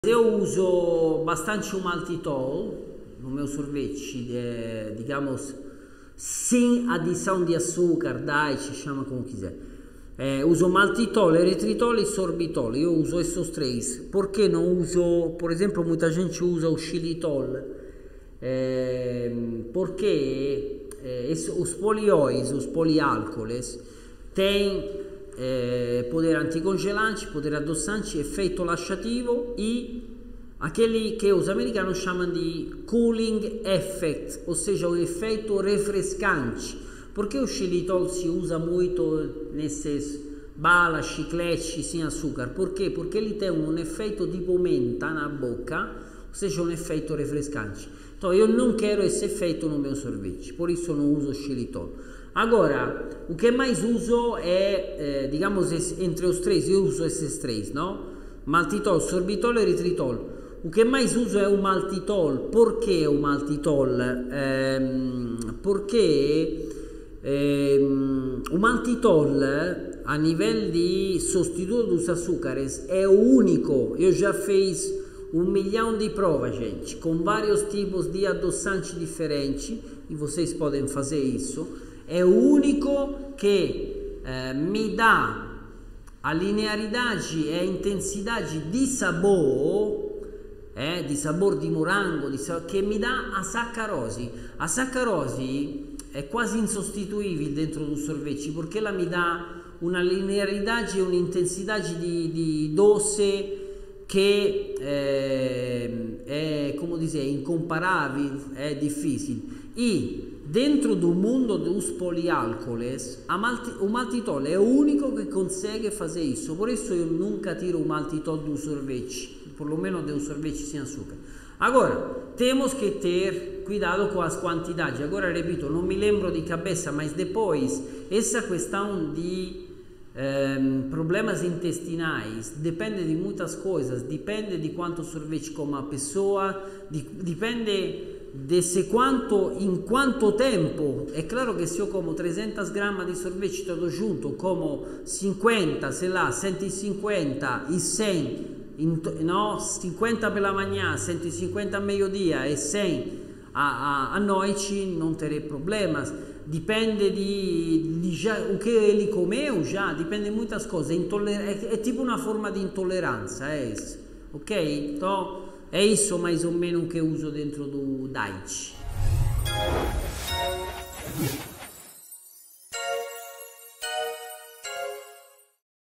Qual o melhor substituto do açúcar para sorvete diet?
Vai fazer sorvete diet? Veja o que usar para não perder o sabor e oferecer um produto delicioso.